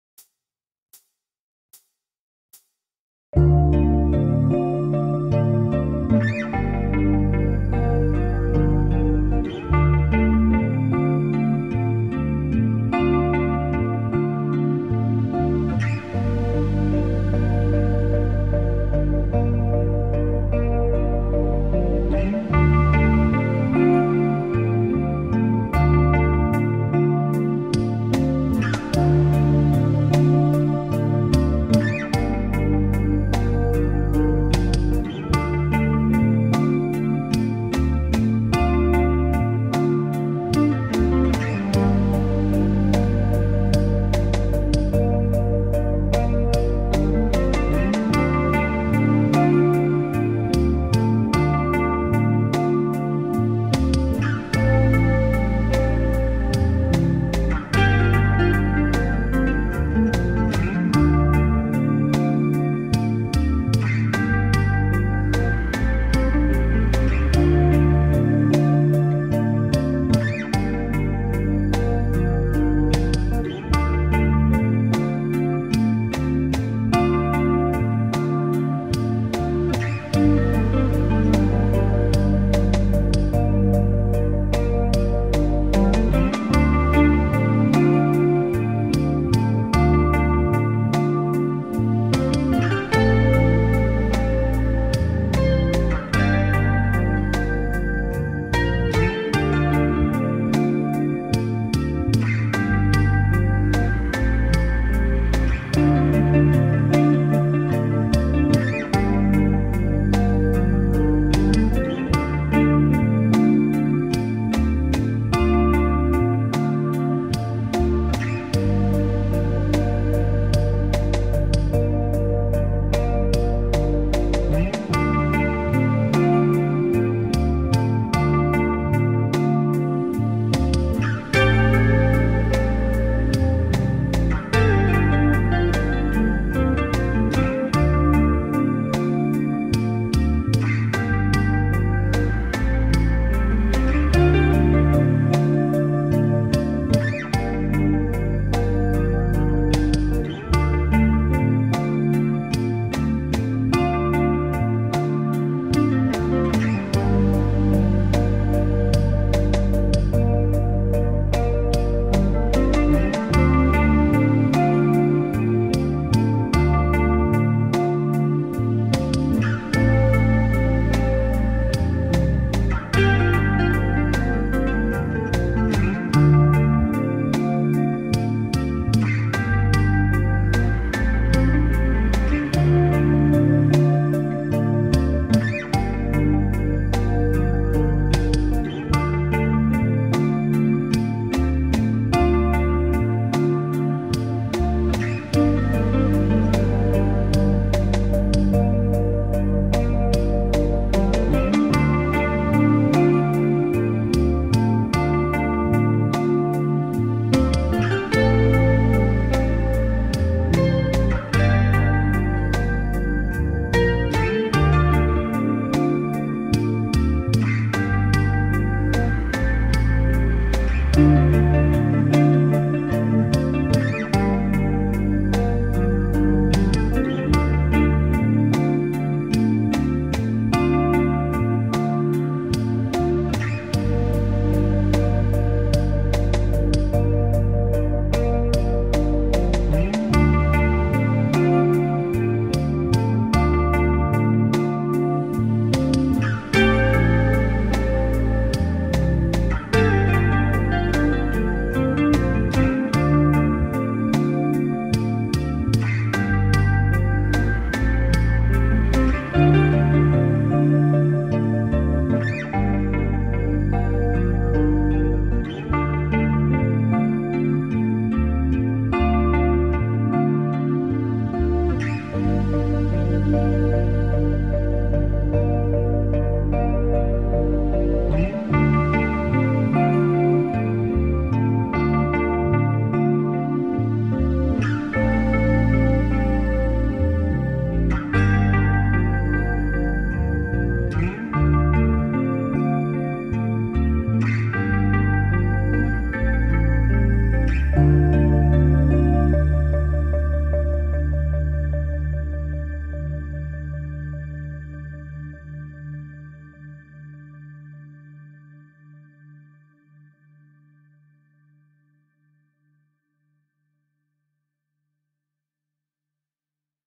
Smooth Atmospheric Blues Backing Track in Dm (D minor)
Hey,

I decided to make yet another backing track. It's in the same style as my Sad Atmospheric Blues in B minor track.

Happy jamming !